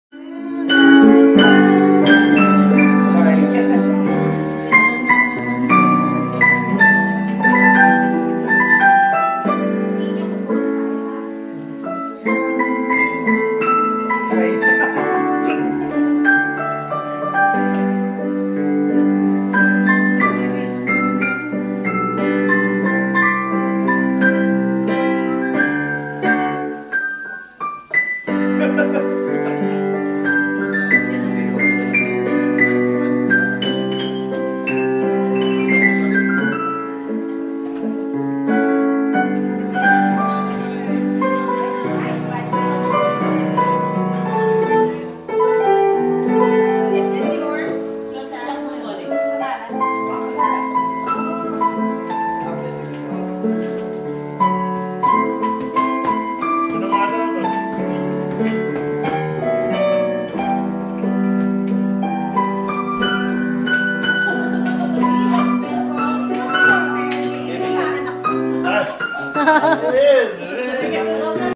ARIA Keyboardist Jerick T with the Philippines' Kuya Mike Villegas
Taken during a private dinner for Aiza and Liza during the High On Love Concert in Chicago